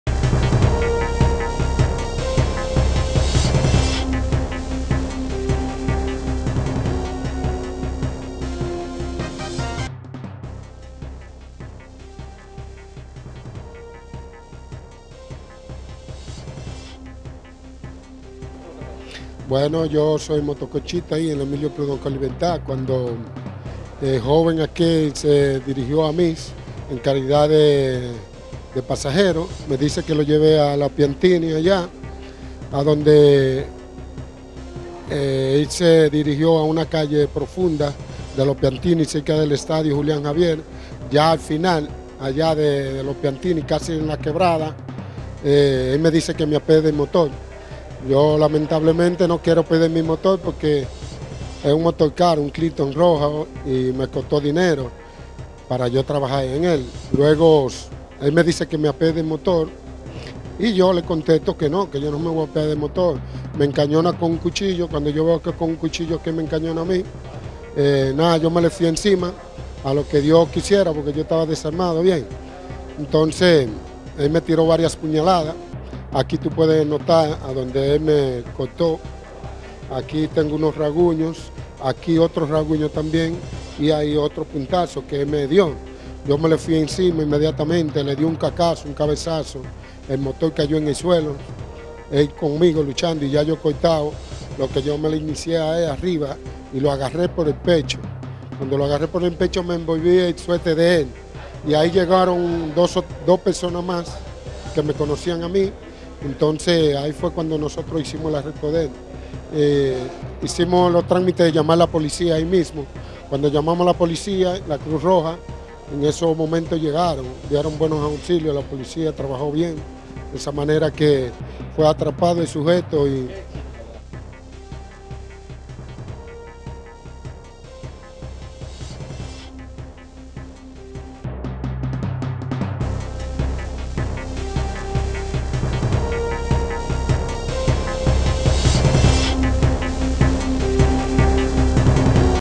Multitud apresa hombre supuestamente intento asaltar otro en SFM
San Francisco De Macorís.- El sábado 16 de noviembre una multitud apreso y golpeo a un hombre, mientras supuestamente intentaba despojar de una motocicleta a un conchero, en un hecho ocurrido en la Urbanización Piantini.

 Rafael Prado narró que el joven tras pedir sus servicios, lo llevó a la urbanización y luego, tras resistirse a entregar la motocicleta, sostuvieron un forcejo donde recibió varios cortes con un arma blanca, y le propinó varios golpes, y tras neutralizarlo, fue atrapado por la multitud y entregado a las autoridades.